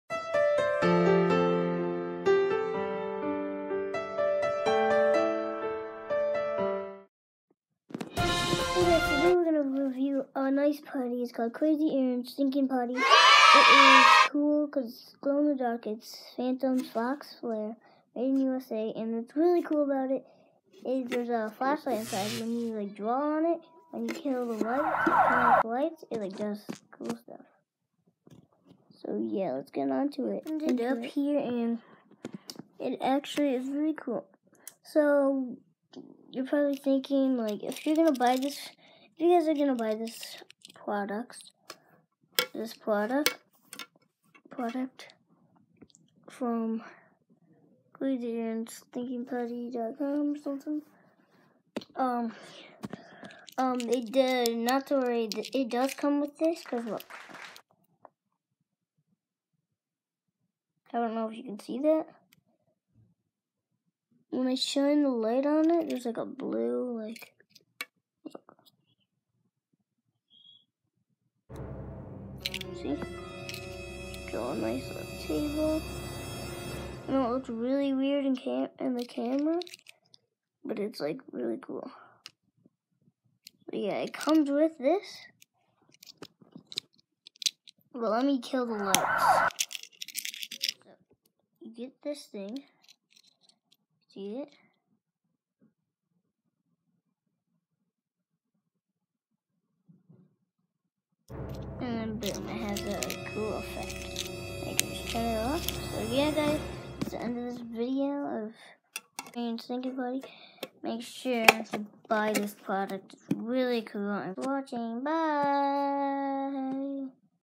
CRAZY Aaron’s ThinKinG PUTTY-Review Foxfire!!
in this video I review CRAZY AARONS THINKING PUTTY it’s awesome easy to clean up and 100% FUN it’s name is foxfire